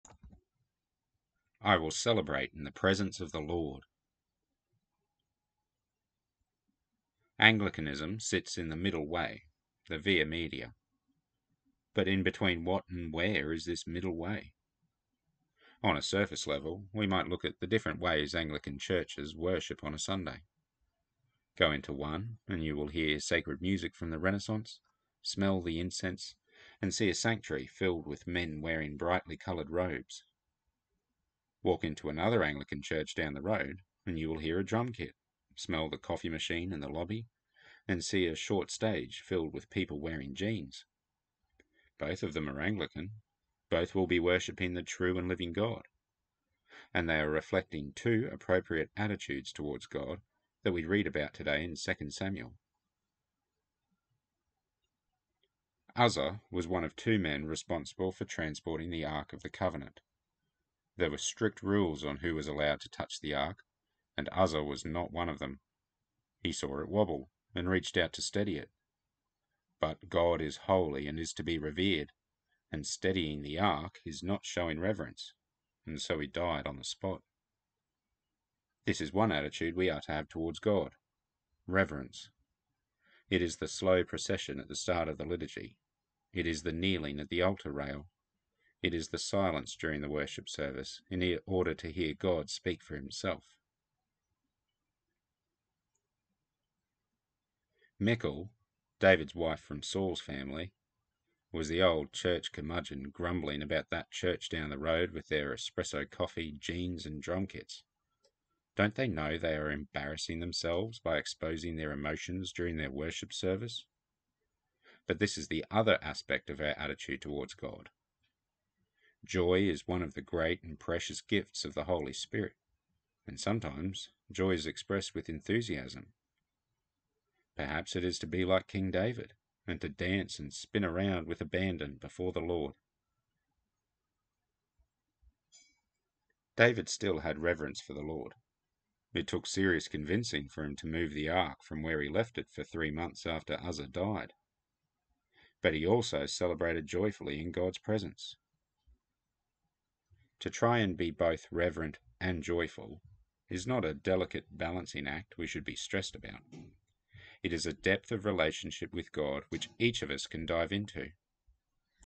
I will celebrate in the presence of the LORD!
Psalm 68:1-20
2 Samuel 6
John 7:32-53

Observance: Brooke Foss Westcott, bishop and teacher (d. 1901); John Comper, priest (d. 1903)

Devotional reflection for the Morning Prayer readings set in the Revised Common Lectionary.

Prefer to read?